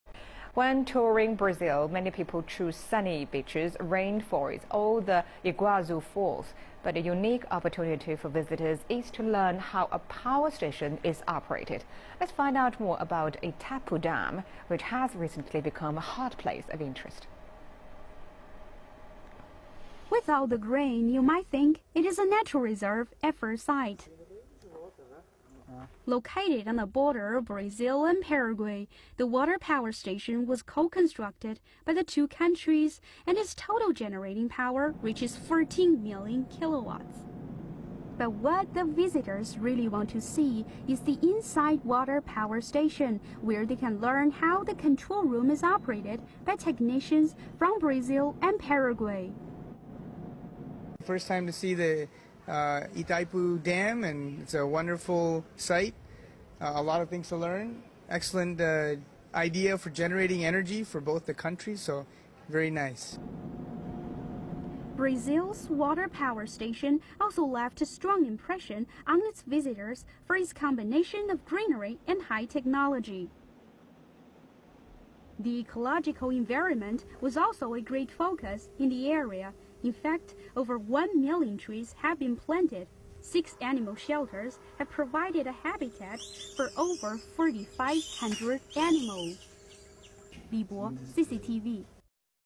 Brazil's water power station opens for tourists
When touring Brazil, many people choose sunny beaches, rain forests or the Iguazu Falls. But a unique opportunity for visitors is to learn how a power station is operated. Let's find out more about Itaipu Dam, which has recently become a hot place of interest.